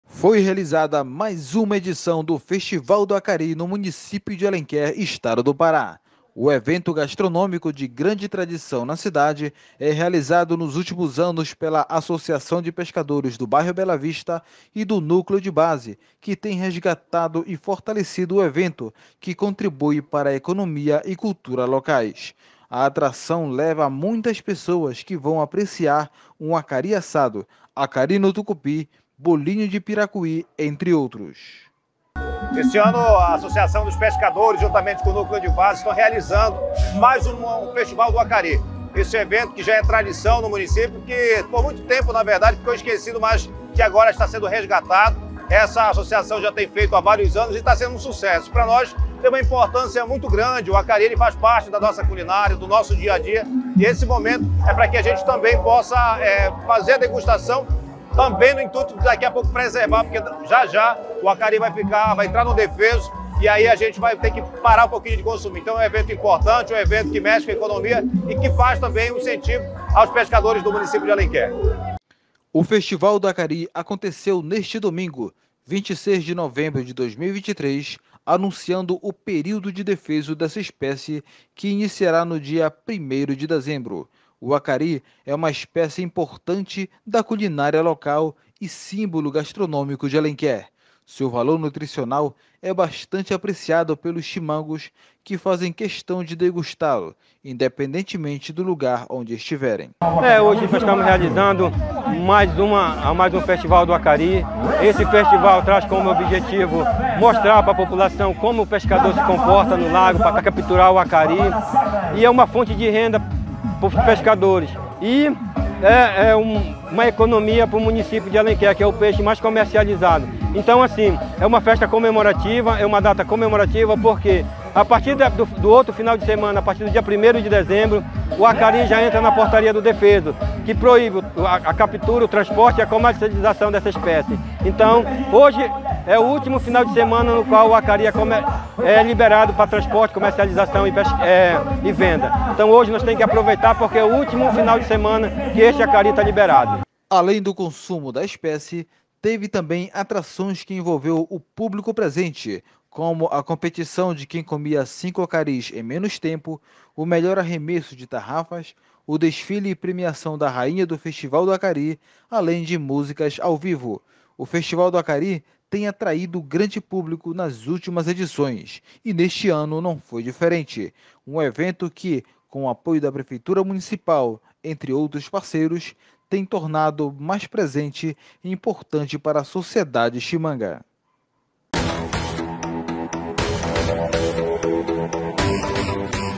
Festival do Acari 2023 - Alenquer PA
Foi realizado neste domingo 26/11 a edição 2023 do Festival do Acari, em Alenquer PA.